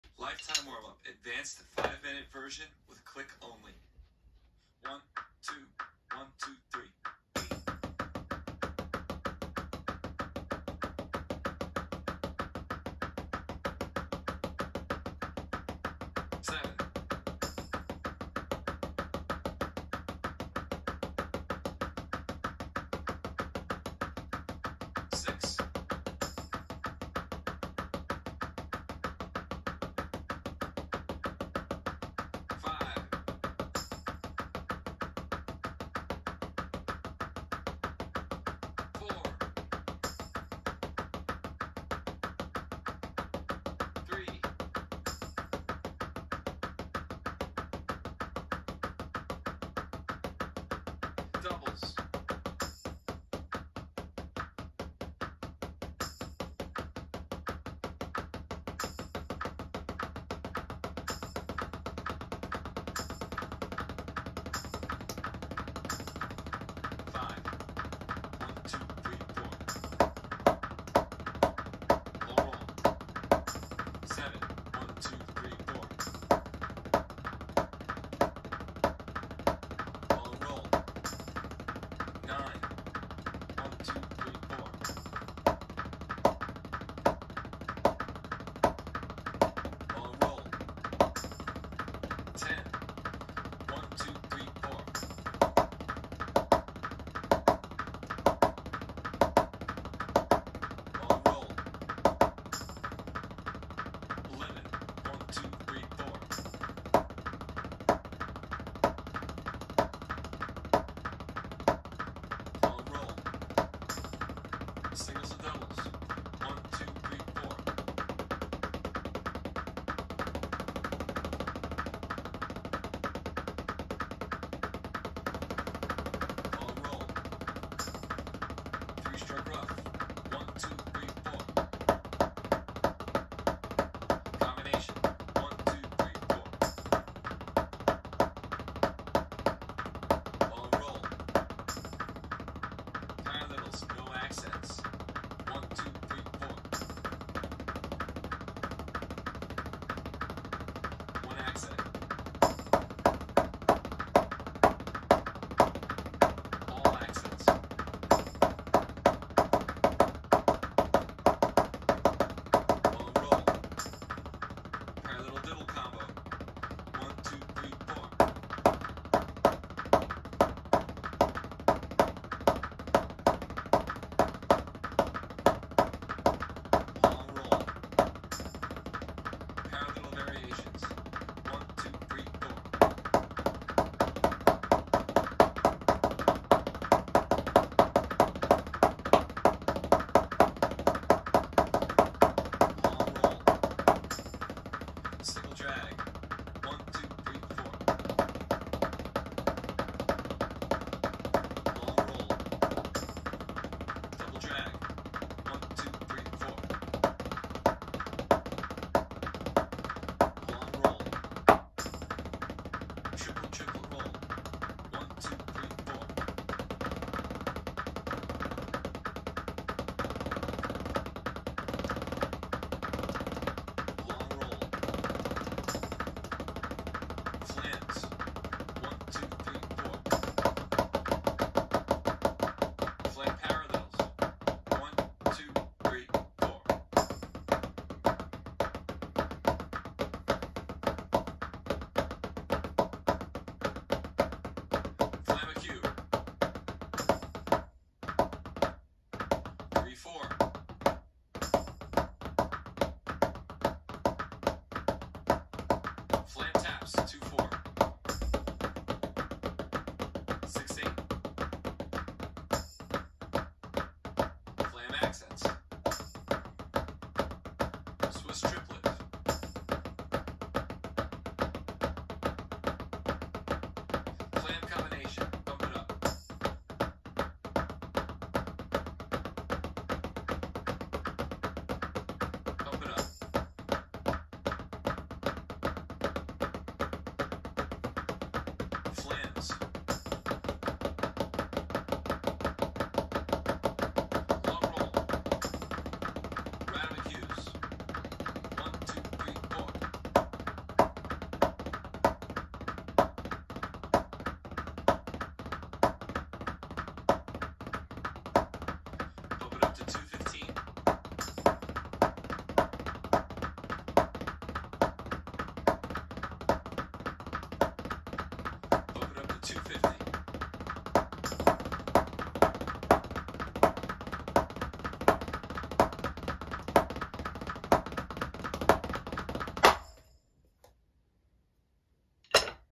Tommy Igoe Lifetime Warm Up - Advanced 5 Minute Version
Thought I’d upload this as I do this or the 10 minute version pretty much every day and before every show I play. It’s one of the most comprehensive warm ups in my opinion and gets your mind ready creatively before playing.

The advanced one is pretty difficult, especially the paraddidle variations and transitioning to the flams.

Follow along, let me know how you get on 🙌